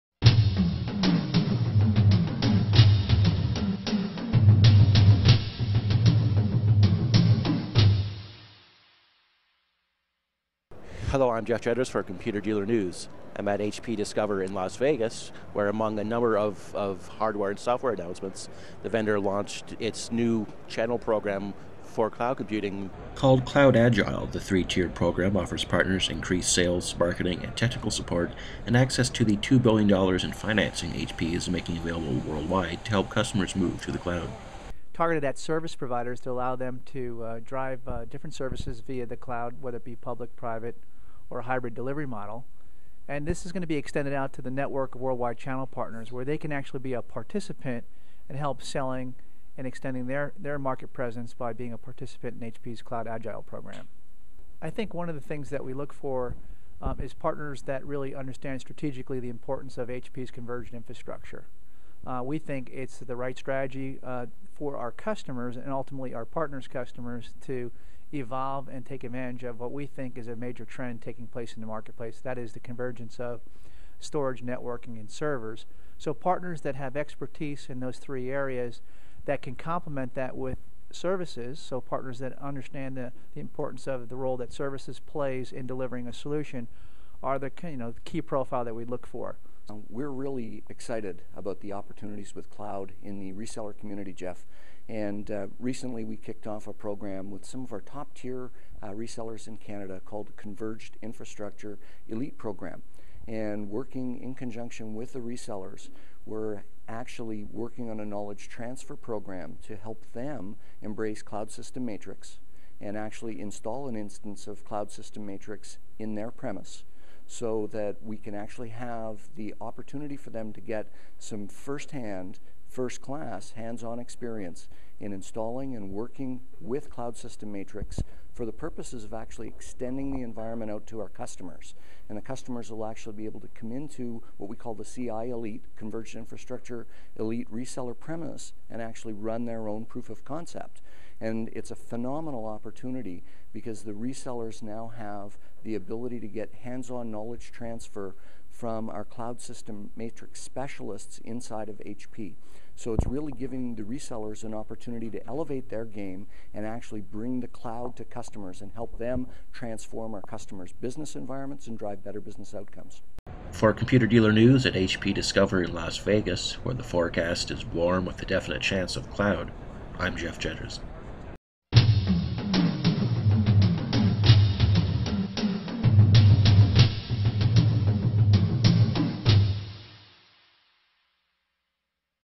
HP's CloudAgile partner program
HP launched its long-awaited CloudAgile partner program at HP Discover in Las Vegas. Jeff Jedras talks to HP executives about the cloud opportunity for HP's channel partners.